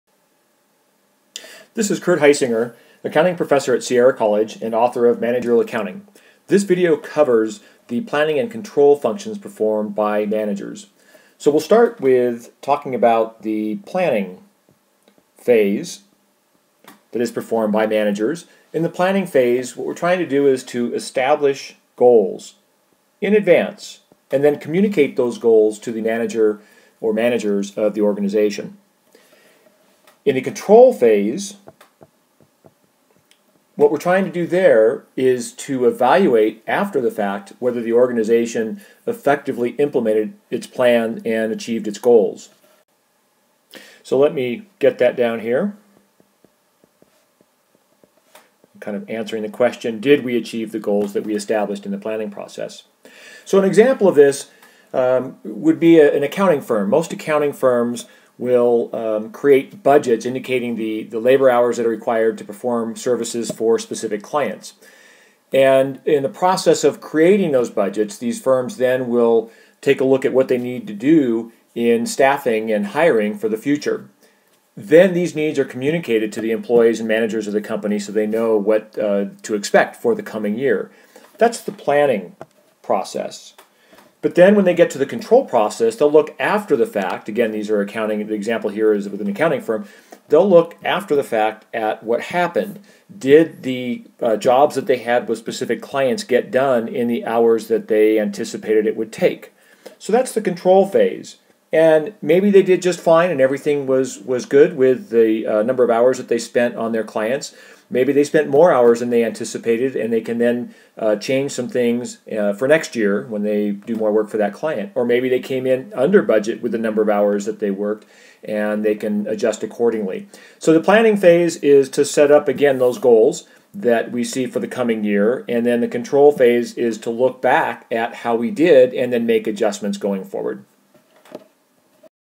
Managerial Accounting 1.2: Planning & Control
This video describes the planning and control functions performed by managers.